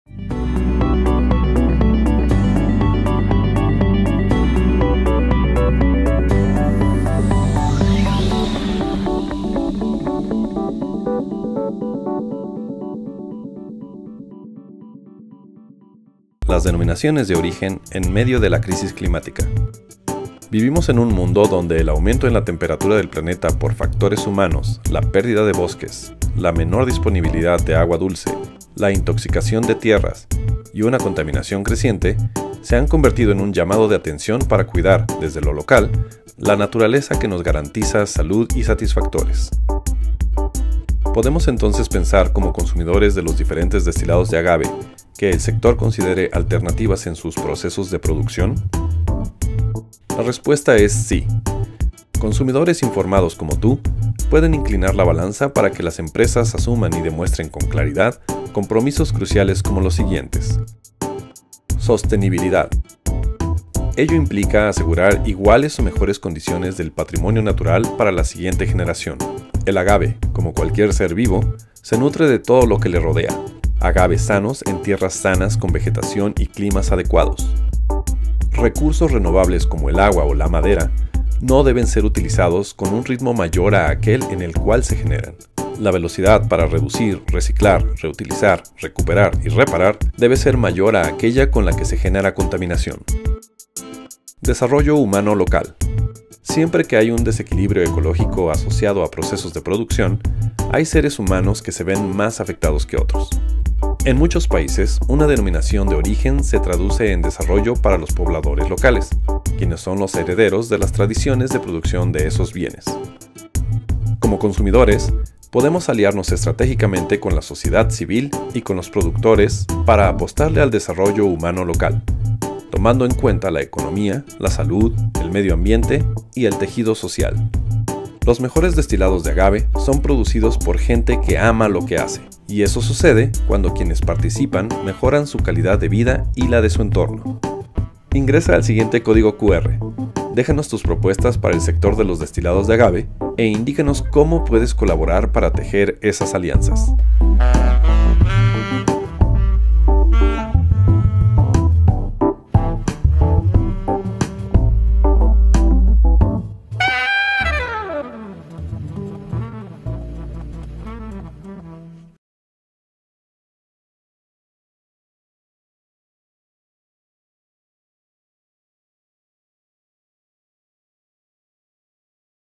Las Denominaciones de Origen en medio de la crisis climática - Vino, Tequila, Mezcal...
🎥 Las Denominaciones de Origen en medio de la crisis climática
🔬Proyecto CONAHCYT 319907 | Ciencia Básica y Ciencia de Frontera
👉 A cargo del Dr. José Hernández Lopez (CEGHColmcih)